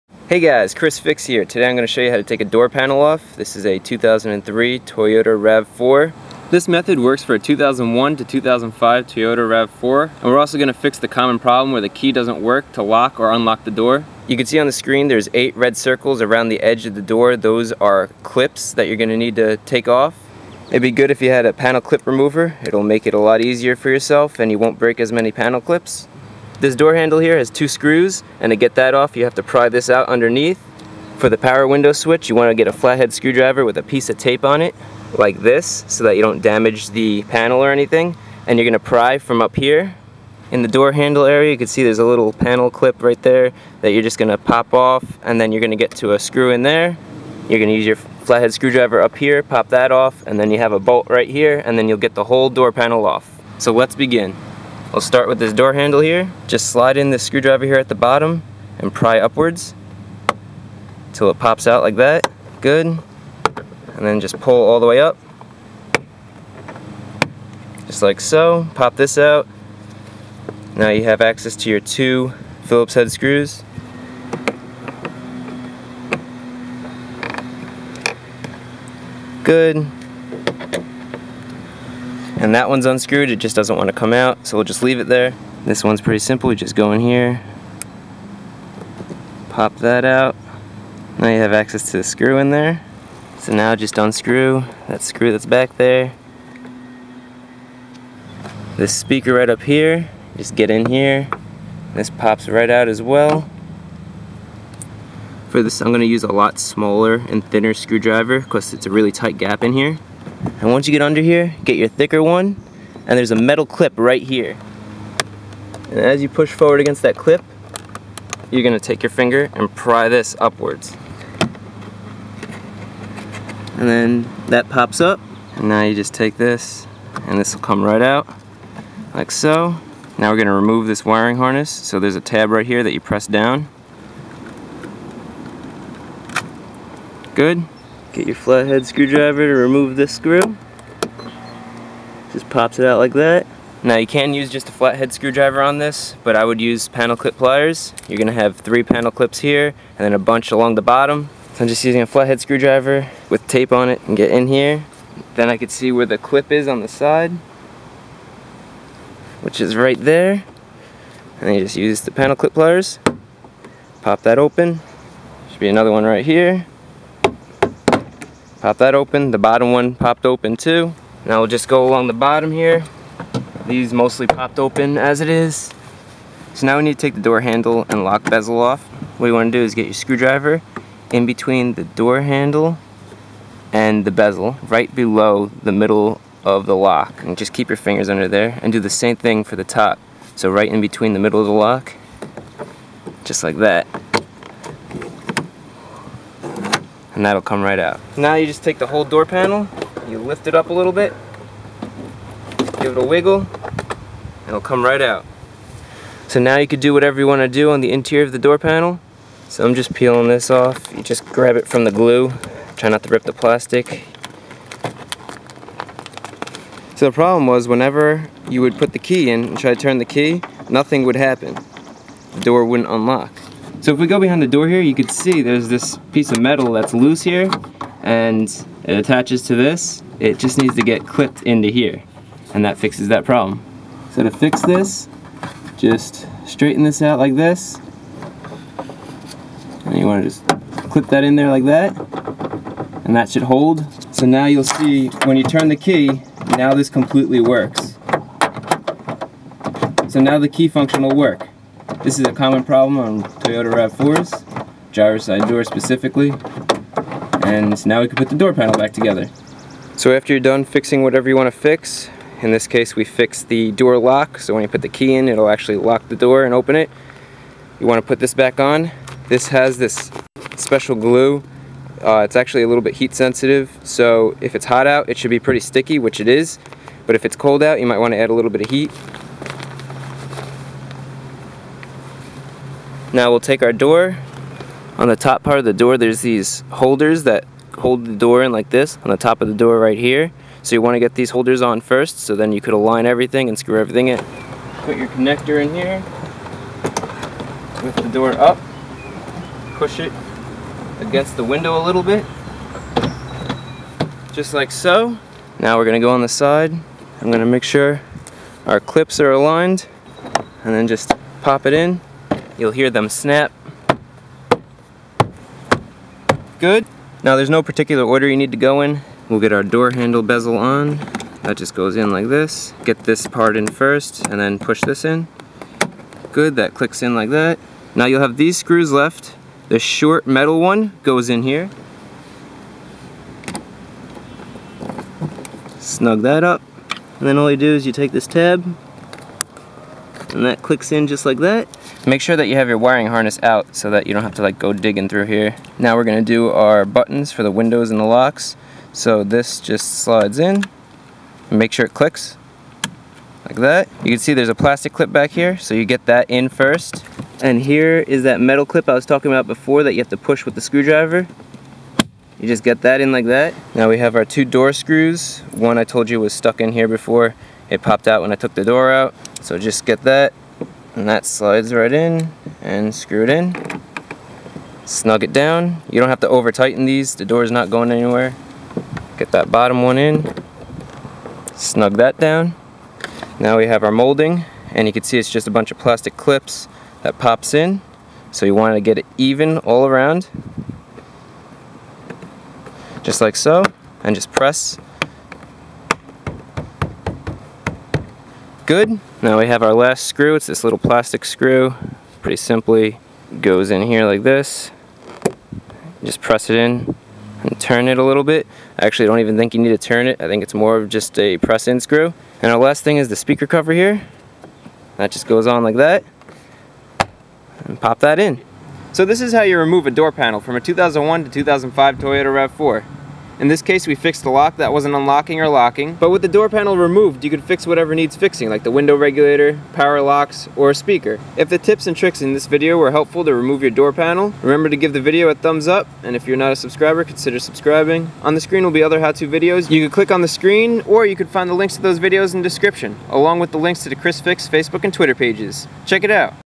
How to Remove a Door Panel in a Toyota RAV4 (2001 to 2005)
Toyota Door Panel Removal and lock fix. Learn step-by-step, how to remove the door panel from a Toyota RAV4. I show you all of the tricks so you dont break anything or damage the door in the removal process. I also show you how to fix the common problem where the door key lock doesn't work.

Toyota RAV4 P1135 and P1155 Fix (Air Fuel Ratio Sensor)